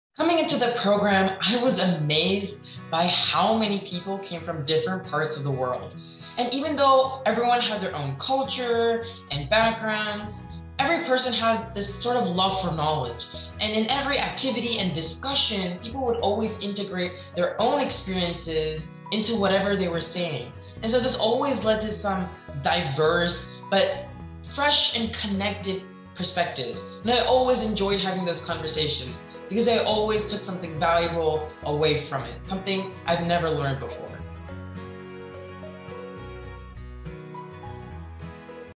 Sharing a Love of Learning with Students from Around the World
Hear about Arnav’s experience in the Harvard Pre-College Program.